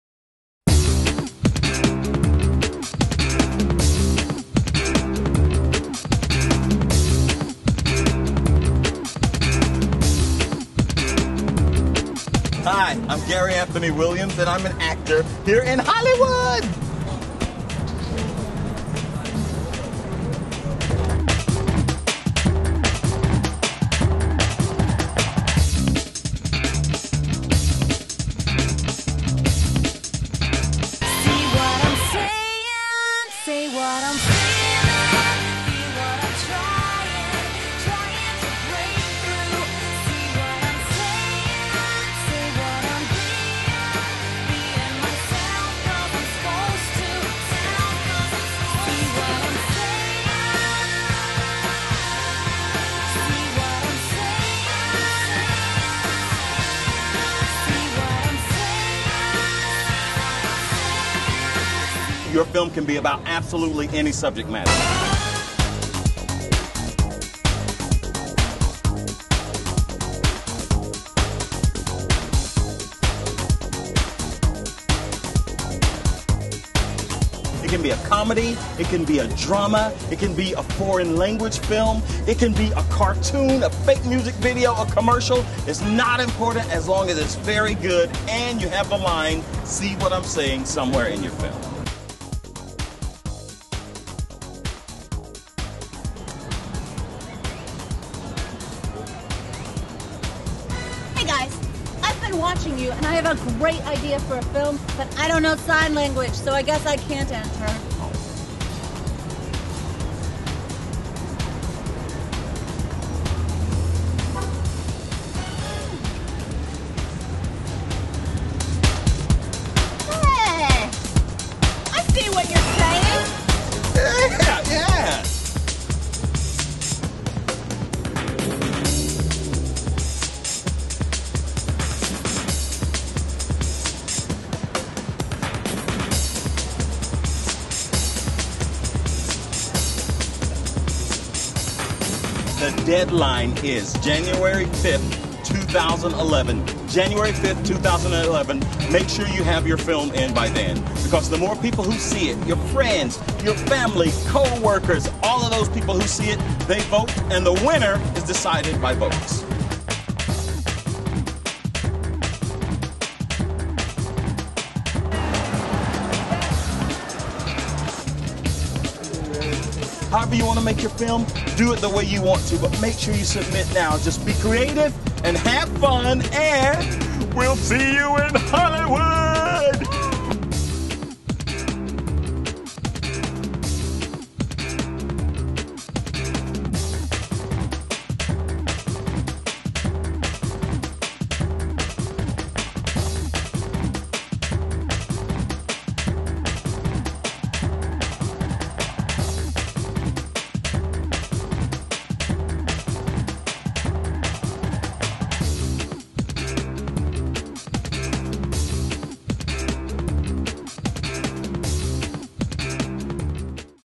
Sprint See What I'm Saying Short Film Contest
Sprint Relay and See What I'm Saying announces short film contest to promote captioned films. It's free to enter - upload your short film up to 3 minutes long that includes the phrase, "See what I'm saying" and win a trip to Hollywood to screen your film and other prizes. The films with the most votes win. Deadline is January 5, 2011.